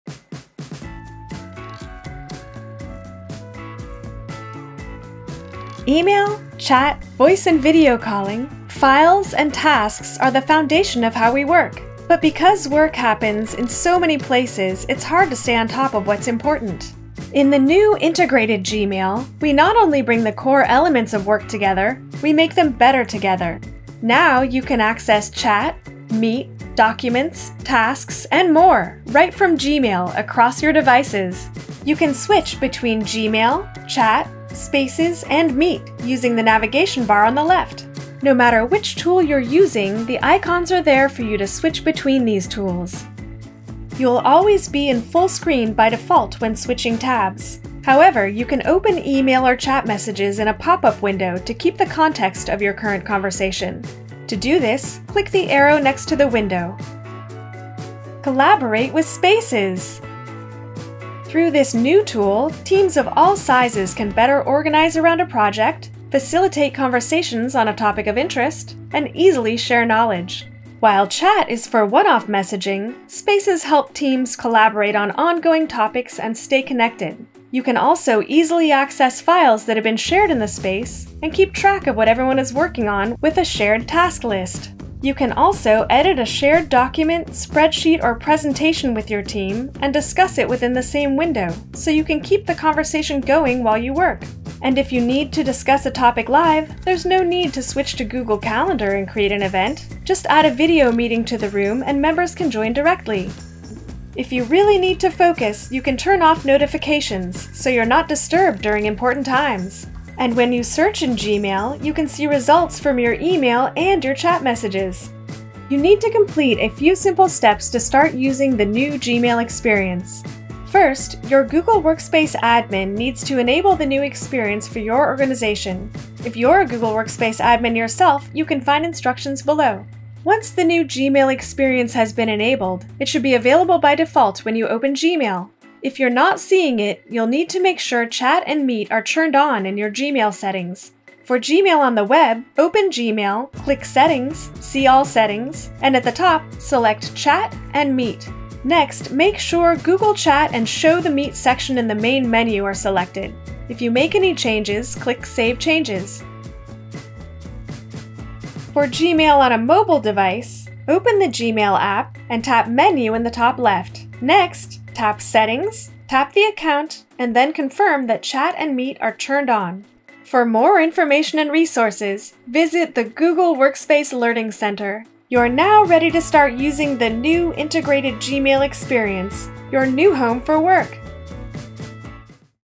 New integrated Gmail quick start for Google Workspace users
Everything you need to get anything done, now in one place: introducing Google Workspace, formerly known as G Suite.

If you use Gmail at work or school, learn how to use the new integrated Gmail experience.

Product: Google Workspace.